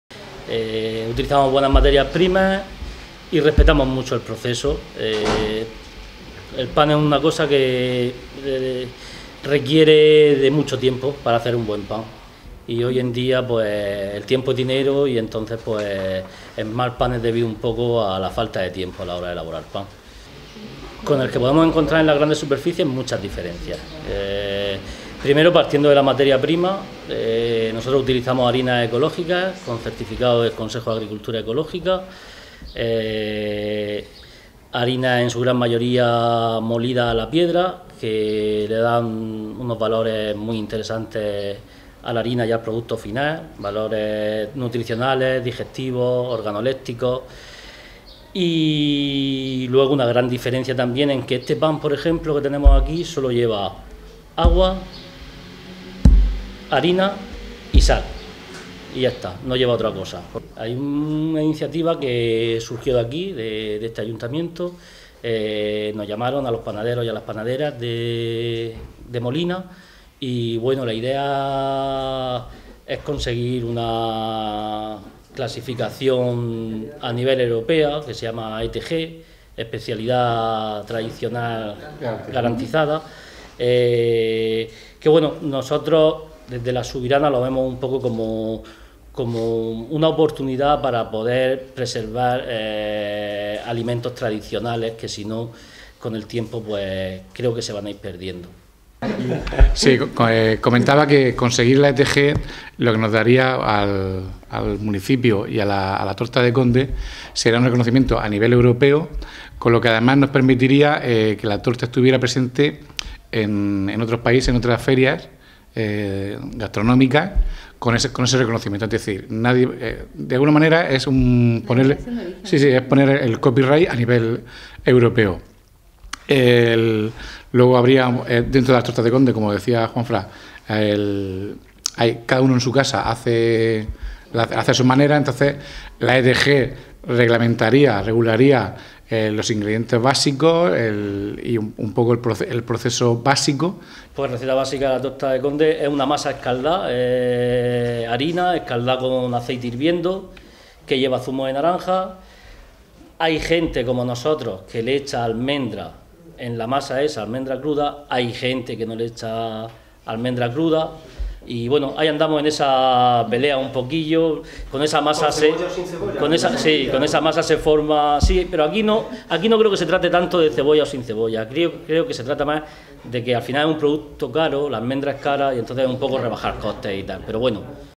Recepción a la panadería molinense 'La Subirana'
El acto de recepción a la panadería molinense La Subirana ha tenido lugar el martes 2 de abril, en el Despacho de la Alcaldía del Ayuntamiento de Molina de Segura, donde la Alcaldesa de la localidad, Esther Clavero Mira, ha entregado una placa de reconocimiento Juan Antonio García García y Juan Ramón Hernández González, socios de La Subirana. También han asistido el Concejal de Economía, José de Haro González, y el Concejal de Artesanía, Andrés Martínez Cervantes.

La Subirana ha entrado a formar parte de esta lista de las mejores panaderías de España, que se elaboraba por segundo año consecutivo con la participación de un jurado compuesto por expertos de la plataforma Panatics y la entidad sin ánimo de lucro Pan de Calidad. En concreto, se han seleccionado los 80 hornos de pan top de toda España y, entre ellos, figuran las dos mejores panaderías de la Región de Murcia, según esta lista: Pan Moreno, que ya entró en esa lista en 2017, y La Subirana, que entra por primera vez en este ránking.

Está ubicada en Molina de Segura, donde se pueden encontrar sus productos en un total de siete puntos de venta distintos, incluyendo el mercado semanal de los sábados en la localidad. El reparto de pan de La Subirana llega también a Murcia capital, donde cuentan con cerca de 15 puntos de venta, incluido El club del Gourmet, de El Corte Inglés. Sus productos se pueden comprar también en otros muchos municipios de la Región de Murcia. Actualmente, La Subirana está trabajando en un reconocimiento a nivel europeo, como es la Especialidad Tradicional Garantizada, (ETG), y en otros proyectos de creación de asociación de panaderos artesanos y generación de eventos para dar a conocer el pan artesanal.

Es una empresa en expansión, considerada de altísima calidad por el cuidado en la selección de productos que utilizan para la elaboración de sus creaciones y la extraordinaria acogida de los clientes.